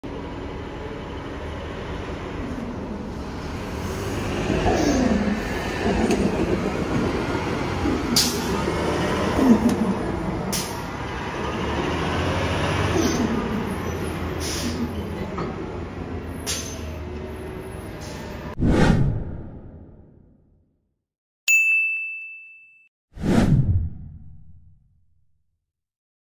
GoRaleigh | 2022 Gillig Advantage CNG (#2204) | Route 20A
This video showcases a 2020 Gillig Advantage CNG Departing GoRaleigh Station.
Route 20A
GoRaleigh Station - White Oaks Shopping Center
GoRaleigh Station
Transfer to GoTriangle Routes 100, 105, 300, 305, CRX, DRX, FRX, WRX, & ZWX.
Video Taken: January 11th, 2024